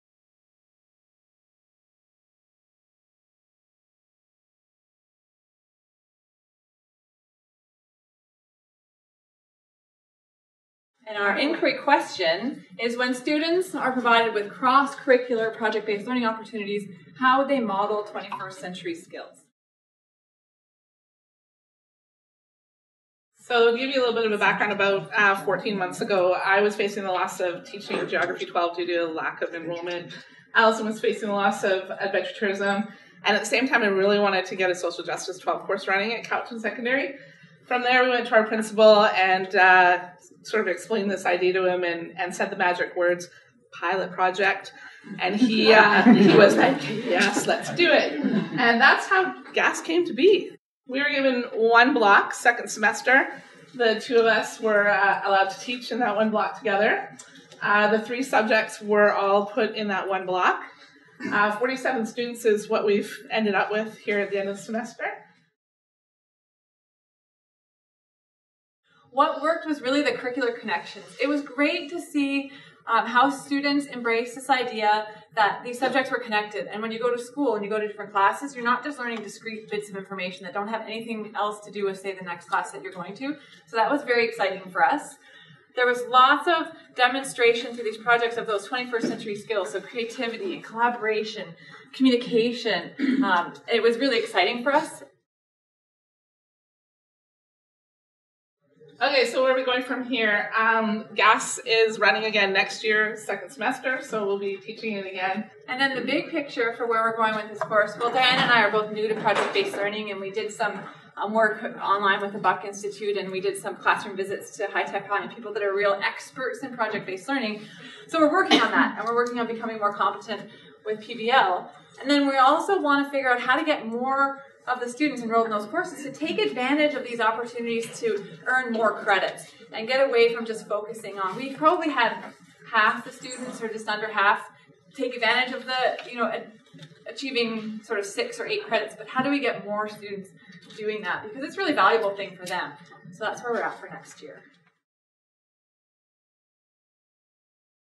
Innovations In Teaching: School District 79 part 2
As part of the changes to the education system taking place in the Province of British Columbia, the Ministry of Education is supporting district-based initiatives that seek to undertake research and inquiry into innovative practices in personalized learning.